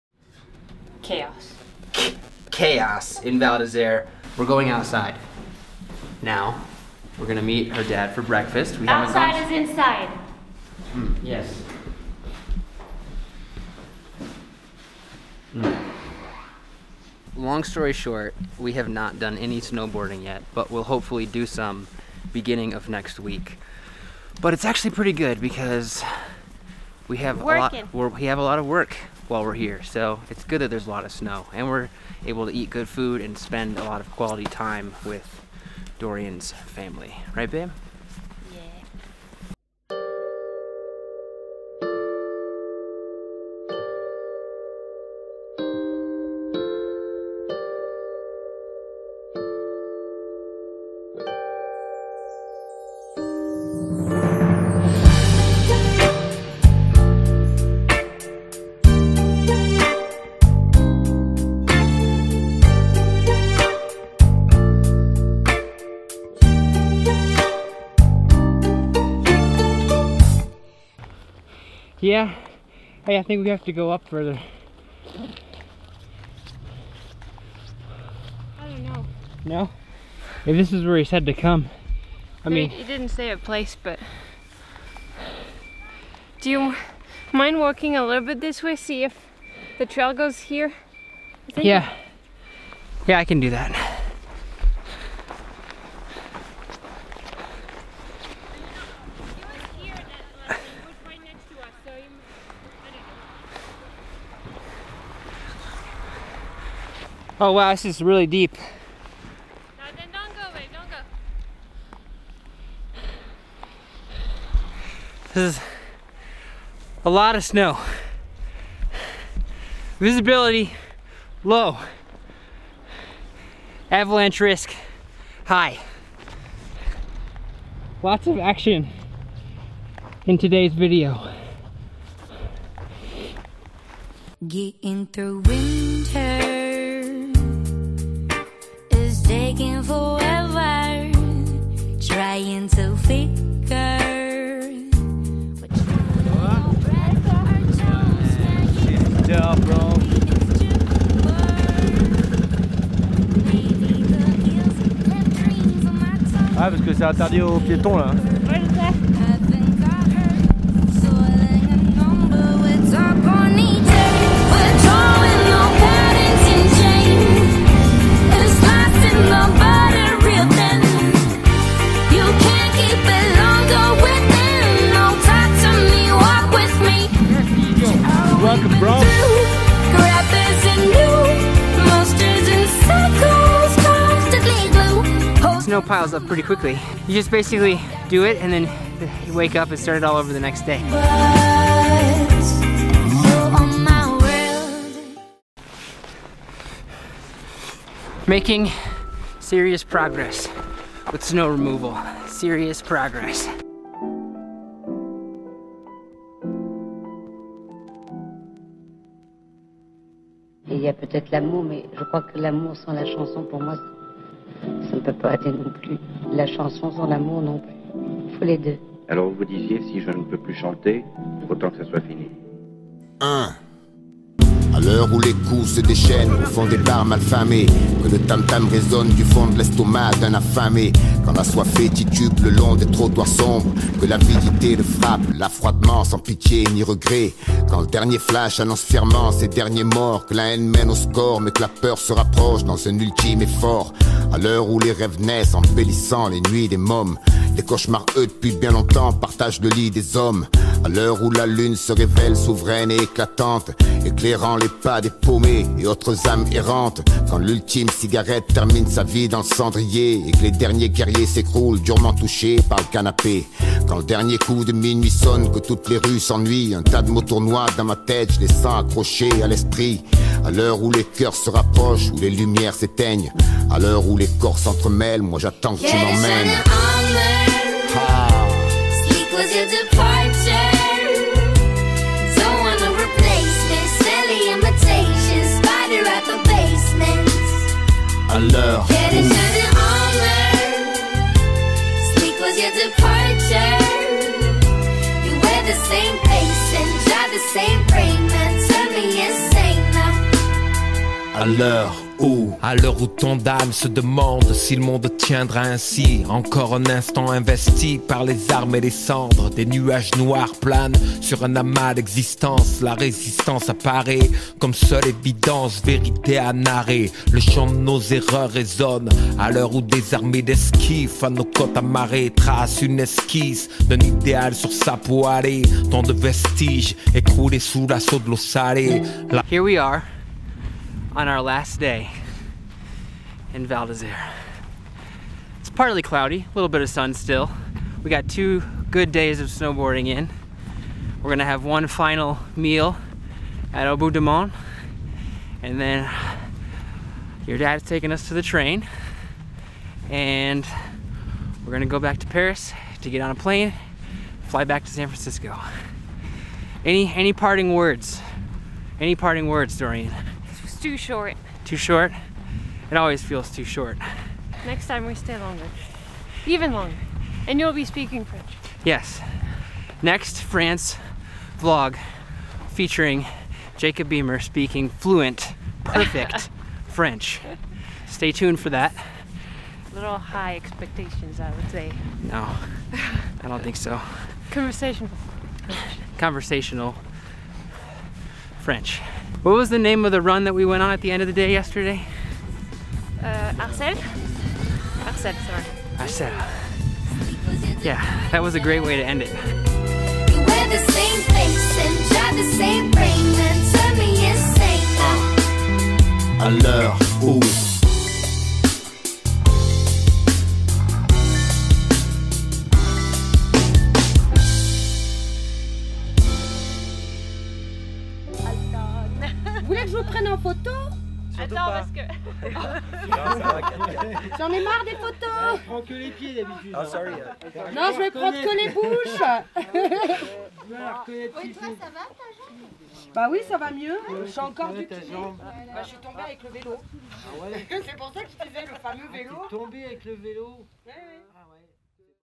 Heaviest snow storm in 30+ years - French Alps 2018
We spent 9 days in the Alps visiting Doriane's family. While there we experienced over 4ft of snow fall in under a week.